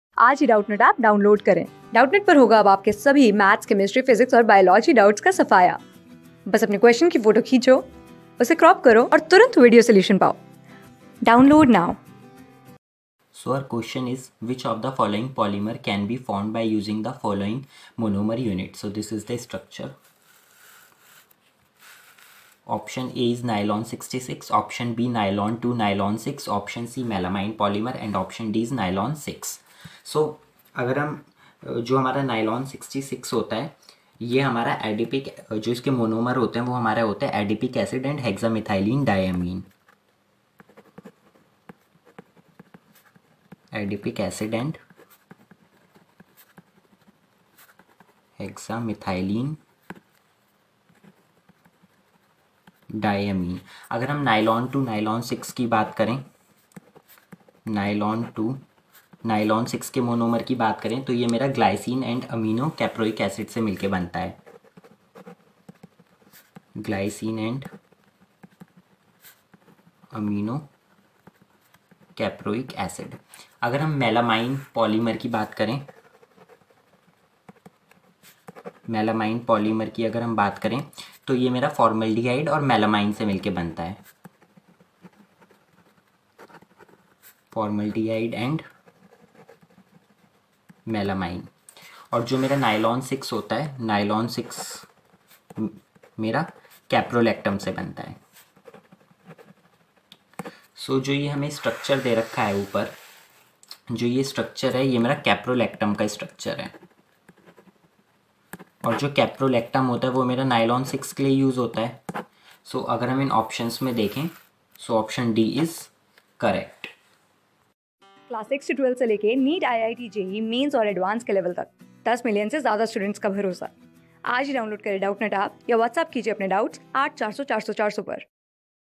Which of the following polymer can be formed by using the following monomer unit? | CLASS 12 | P...
Which of the following polymer can be formed by using the following monomer unit?
Class: 12
Subject: CHEMISTRY
Chapter: POLYMERS
Board:CBSE

👉 Have Any Query? Ask Us.